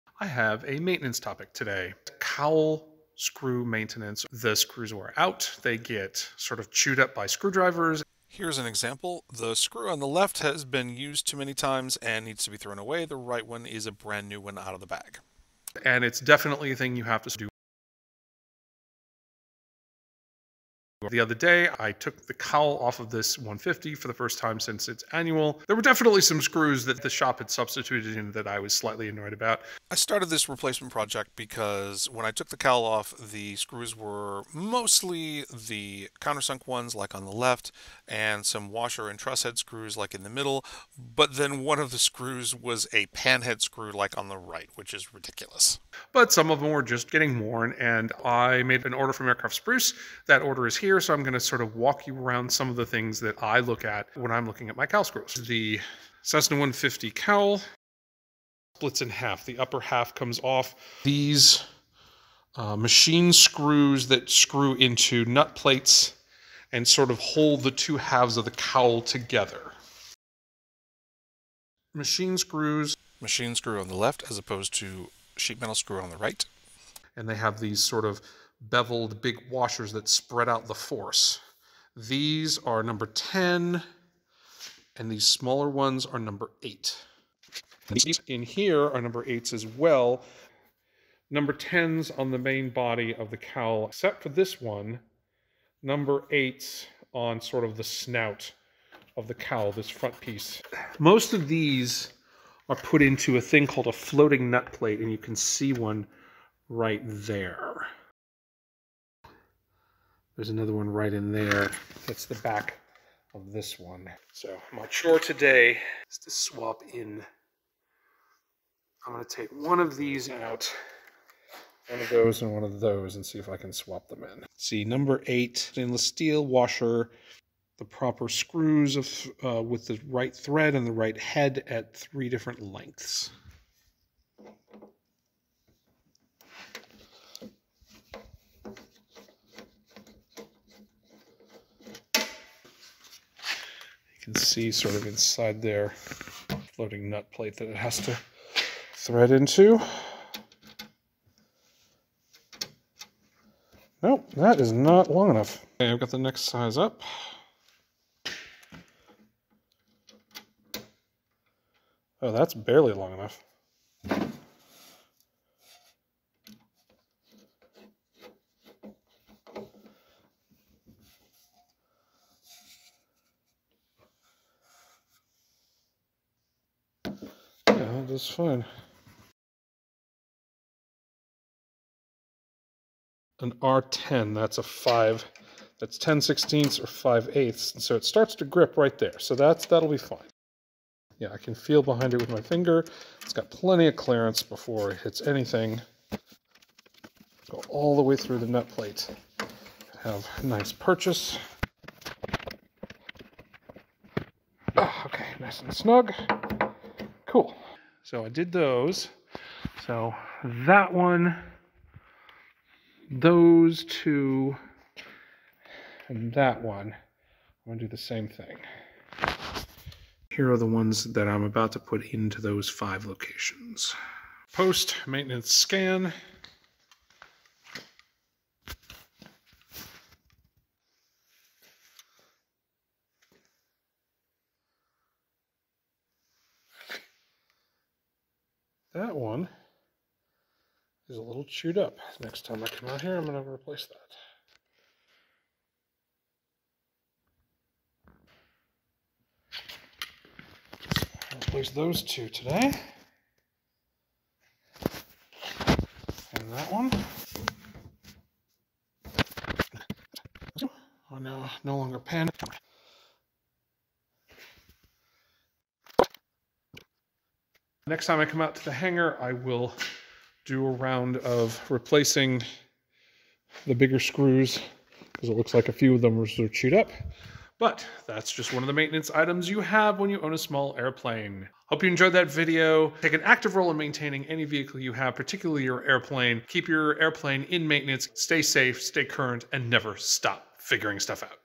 Cowl screw maintenance on my Cessna 150
I swapped out some cowl screws on my Cessna 150 the other day.  It's a periodic maintenance task that's good to do every once in a while.